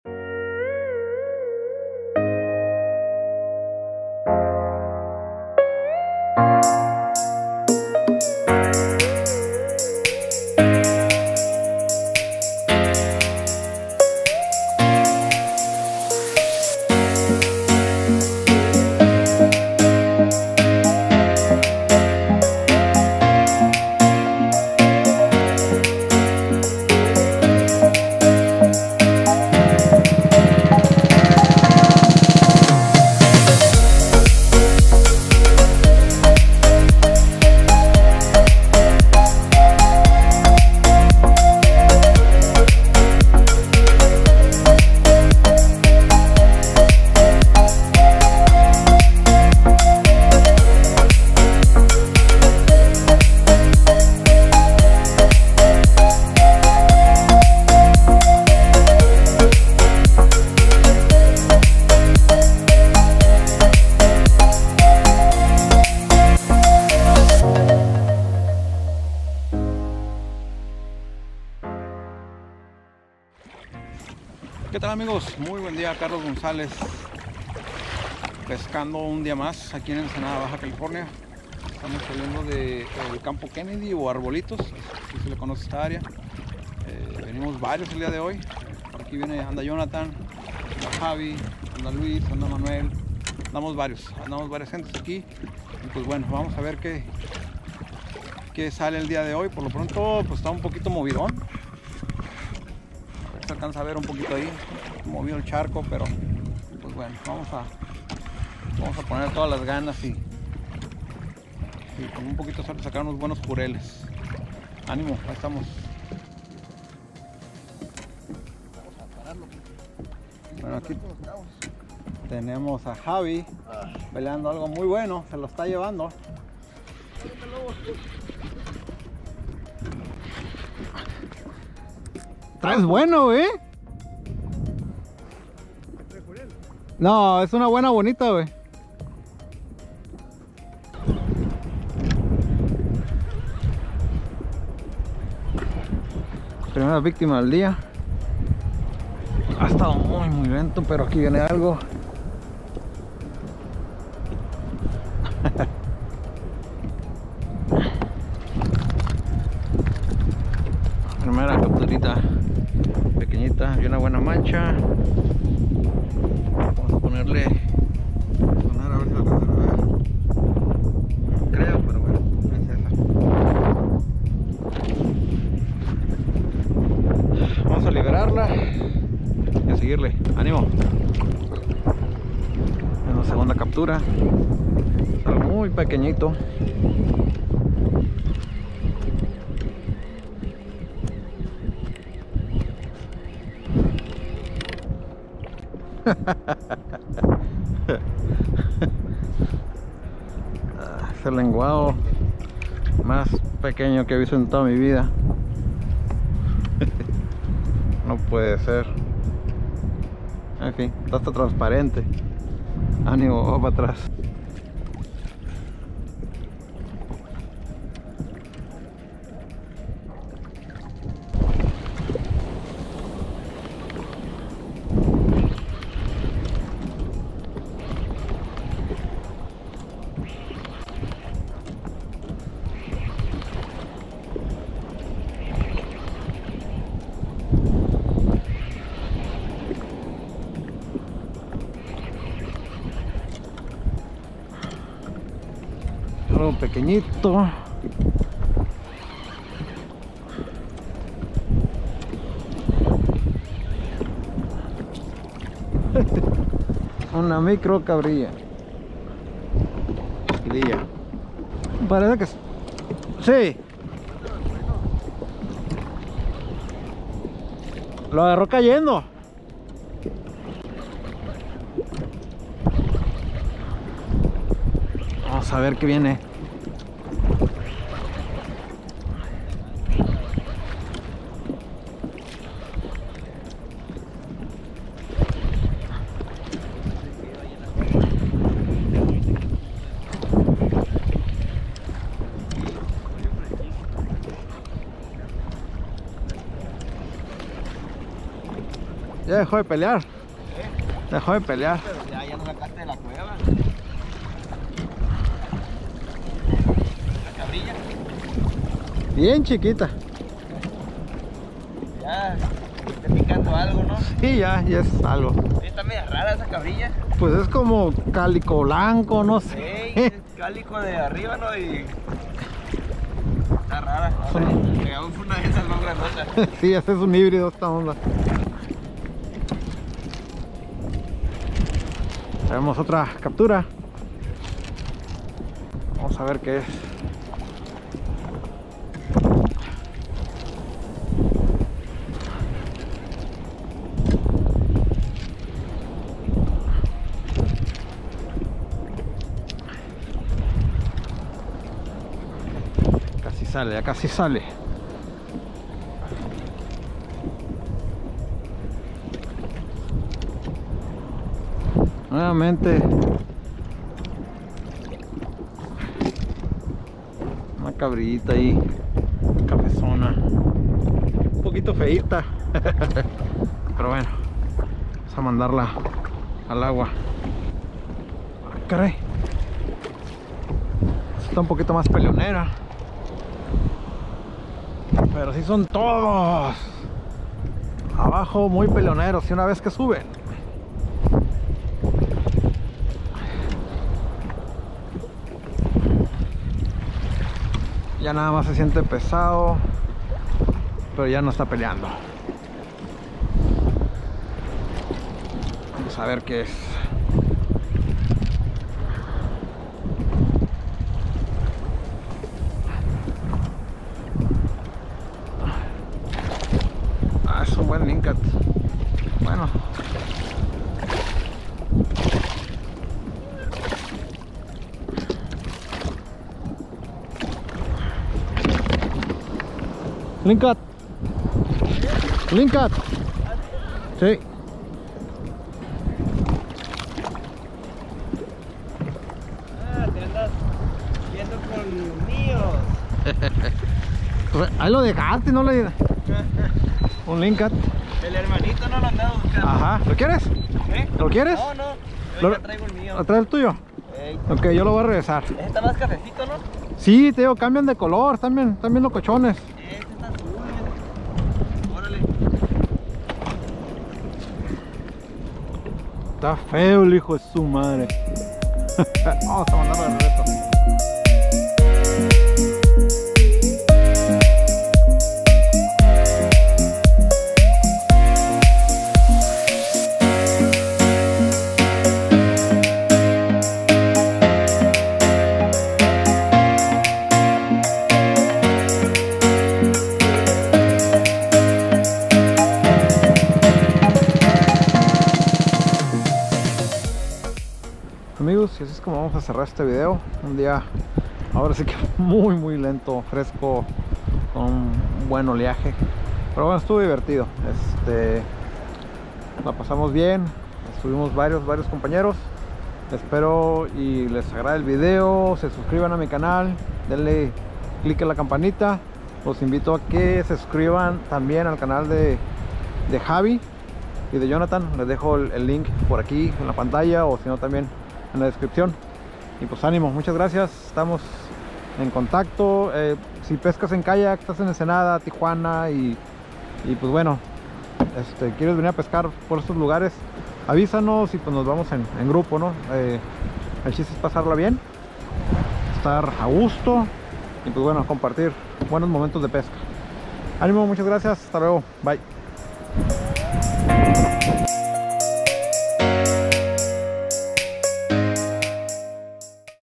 PESCA EN KAYAK- Pescando en arbolitos Ensenada B.C.
Amigos salimos a pescar en arbolitos Ensenada Baja California, el día estuvo fresco y el agua un poco movida, no salieron los animales grandes, sin embargo, el paisaje, unas capturas chiquillas de profundidad y el cotorreo hicieron el día.

Aquí compartimos experiencias, tips y todo lo relacionado con la pesca desde kayak.

saludos a todos y buena pesca!